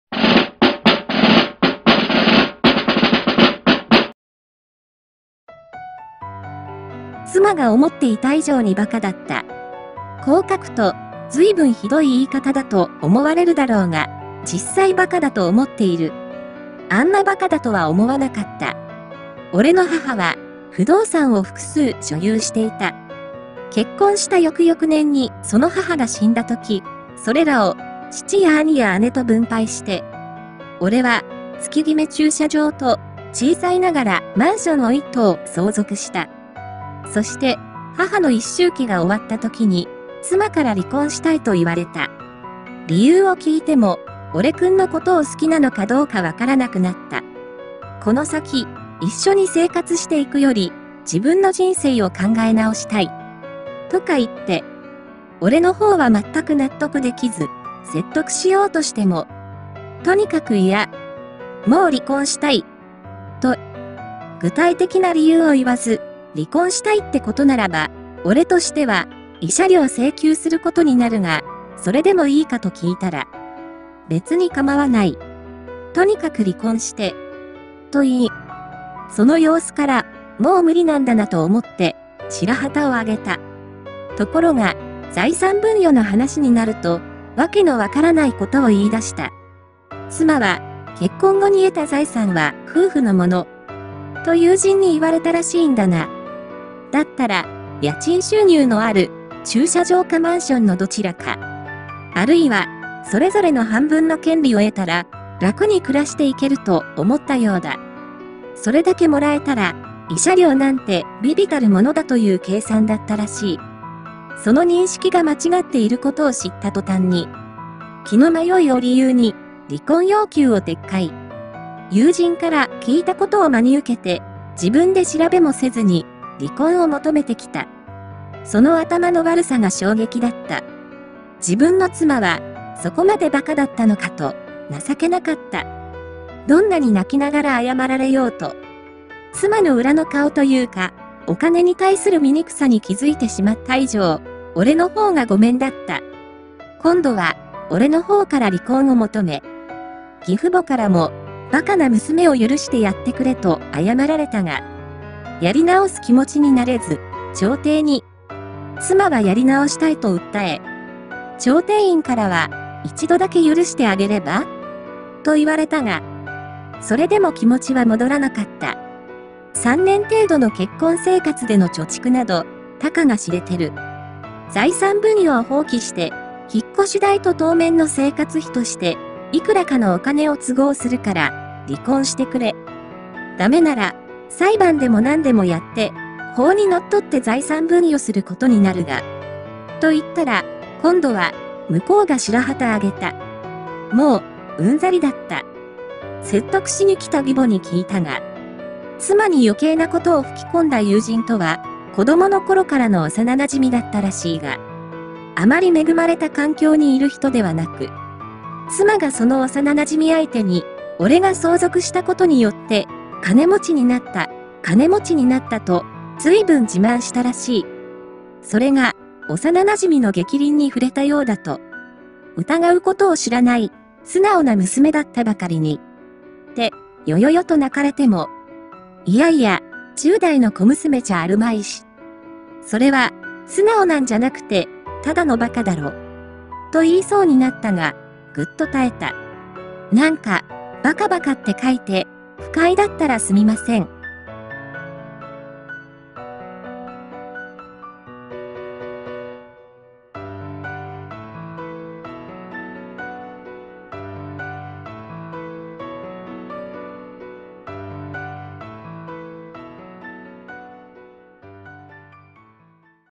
妻「離婚したい。自分の人生を考え直したい」俺「は！？理由は？」妻「とにかく嫌！離婚したい！」俺「・・わかった」→妻「やっぱ無し！離婚しない！」俺（ここまで馬鹿だったとは・・）【修羅場】
DOVA-SYNDROME